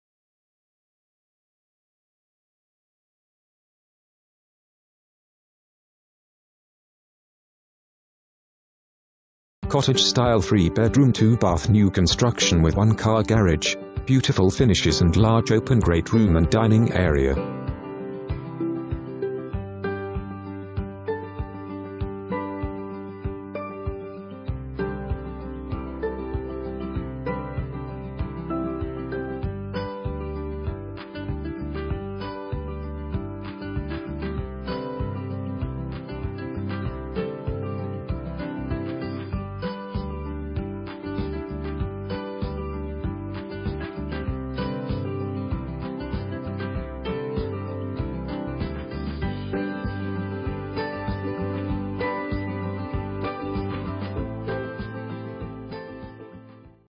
2703 Barkley Drive
3 bed, 2.00 bath, 1,064 SF

Cottage style three bedroom two bath new construction with one car garage, beautiful finishes and large open great room and dining area.

2 full bath

Matthew Chesson
Chesson Property Management, LLC